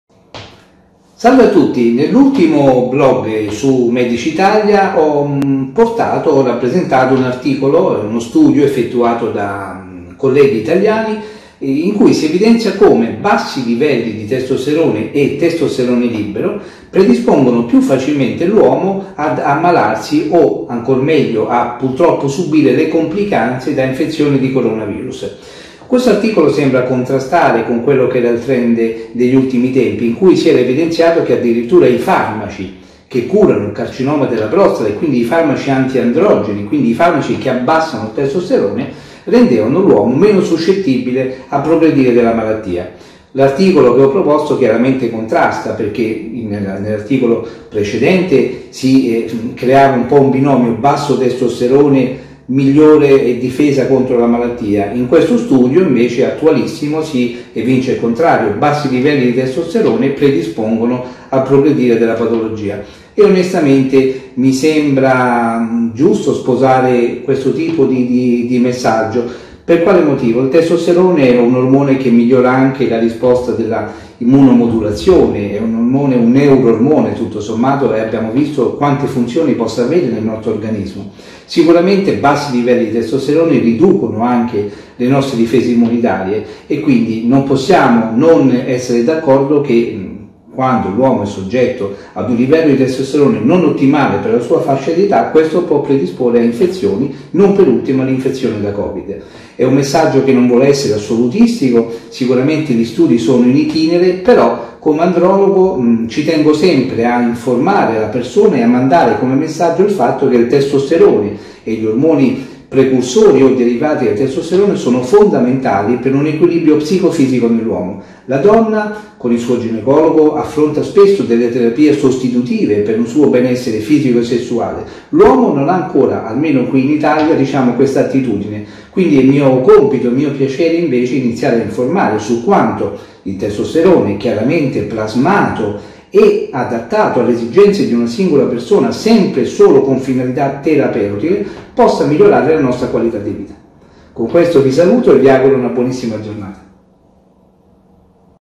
Bassi livelli di testosterone predispongono alle complicanze da Covid
E' quanto dimostra un recentissimo studio tutto italiano
covid 19